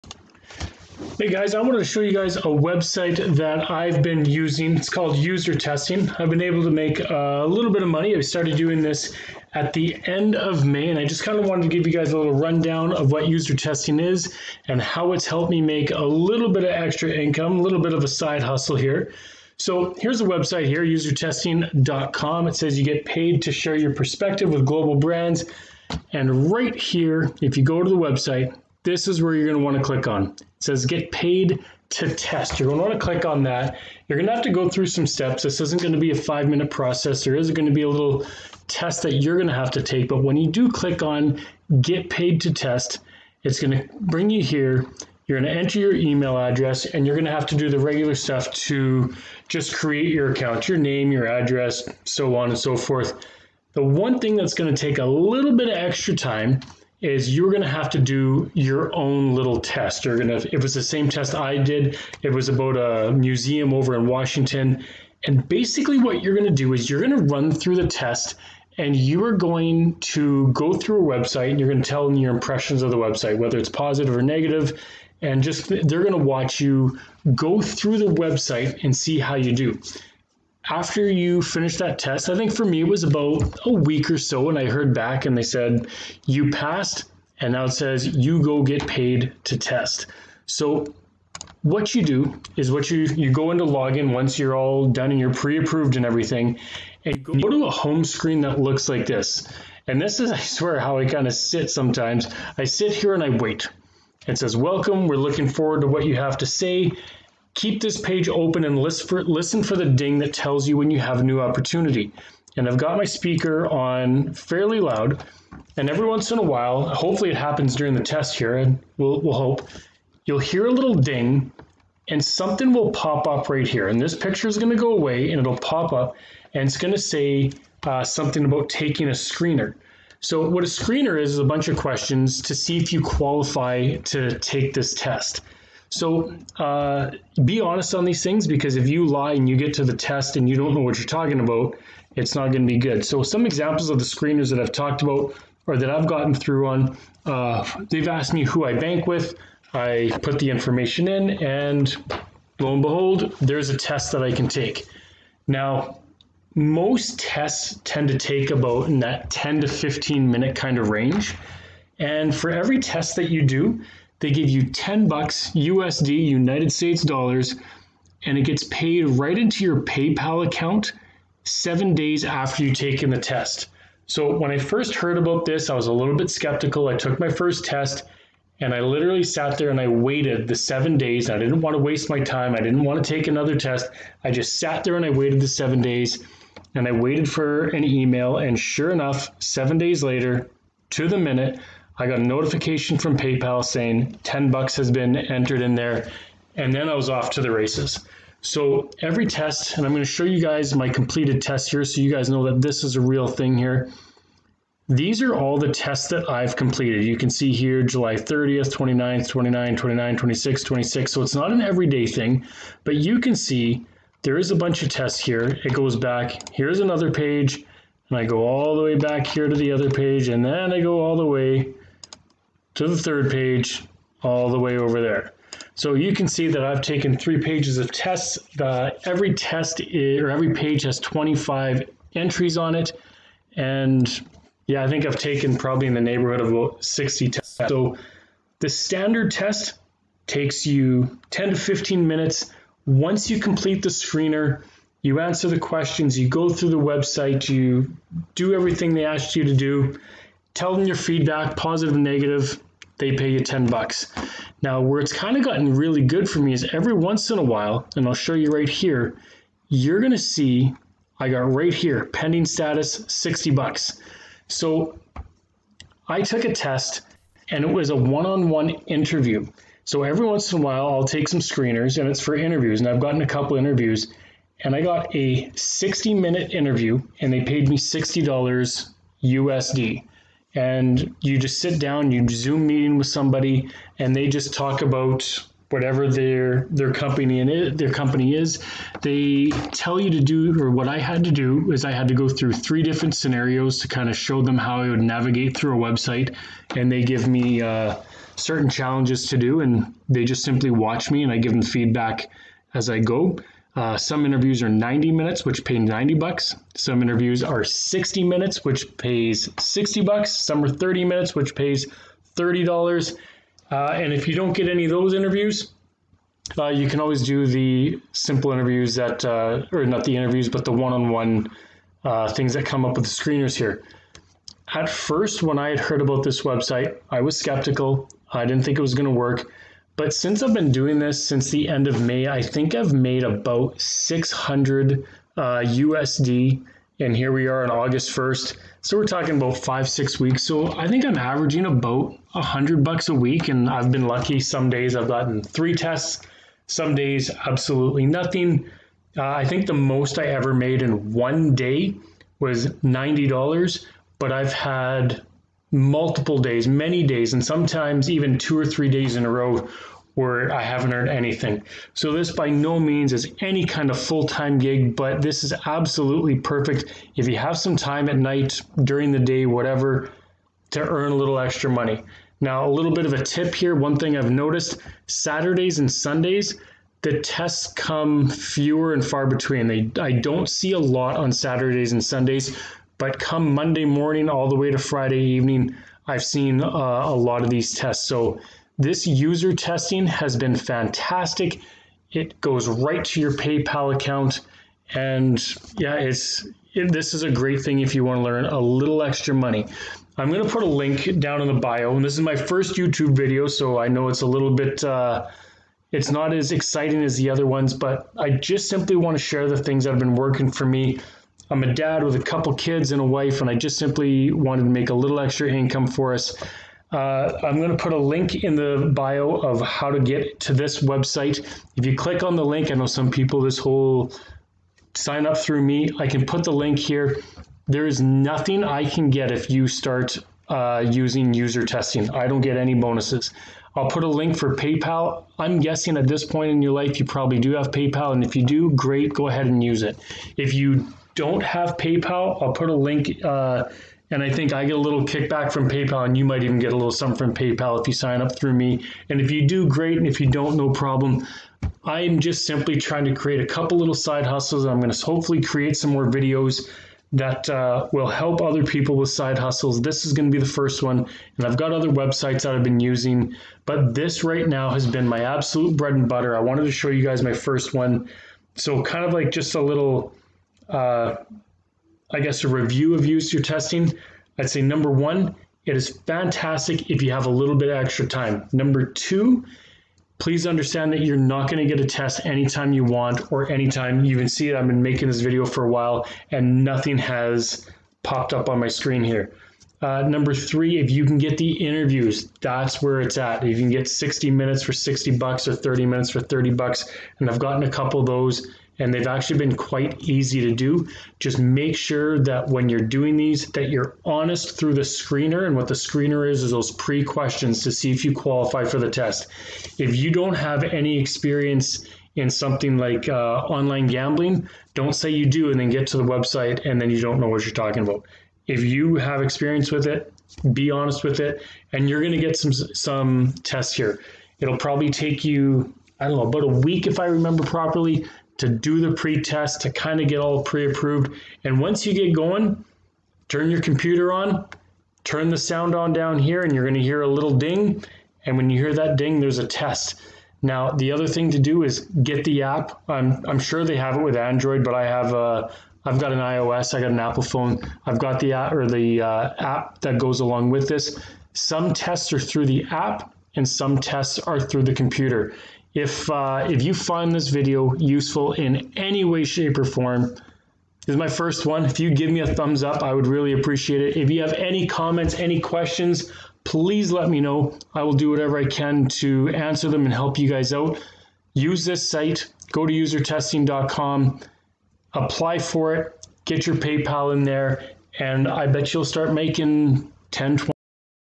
Usertesting Review, make 10 dollars in 10-15 minutes
Quick review of the website UserTesting.com

This was my first video, I'm sure I will learn some editing skills along the way.

I have been using this website since then end of may and have made some extra $$$ with it.

If you use the link, we can both get a 10 dollar bonus.

Please ask any questions, I would love to help!